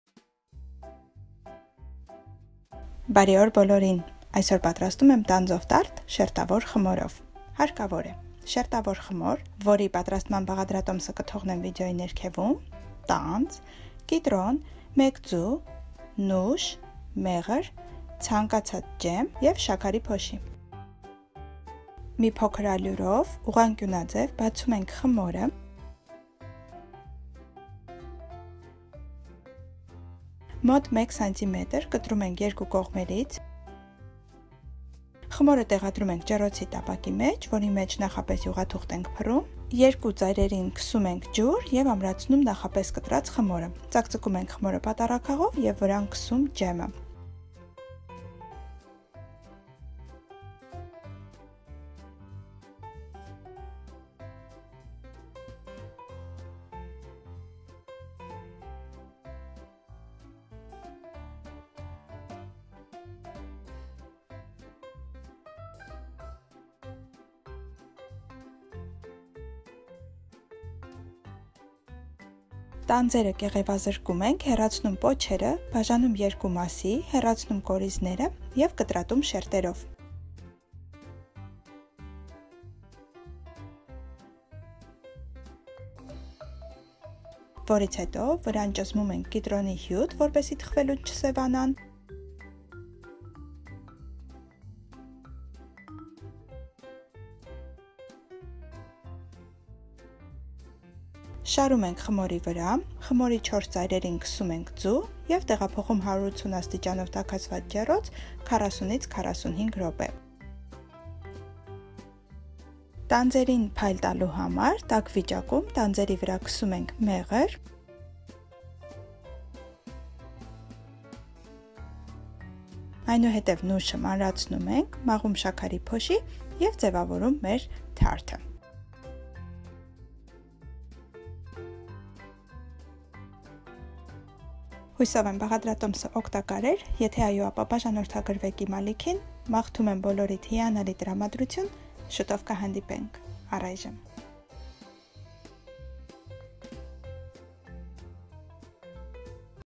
Տանձով Տարտ Շերտավոր Խմորով - Համեղ է
Պատրաստում եմ գեղեցիկ եւ համեղ տանձով տարտ կամ թարթ :)

տանձ
կիտրոն
1 ձու
նուշ
մեղր
ջեմ ձեր նախընտրած
շաքարի փոշի

Պատրաստեք, եւ նշեք ինձ Instagram-ում համեղէ, որպեսզի ես բաց չթողնեմ Ձեր հրապարակումները: @Համեղ է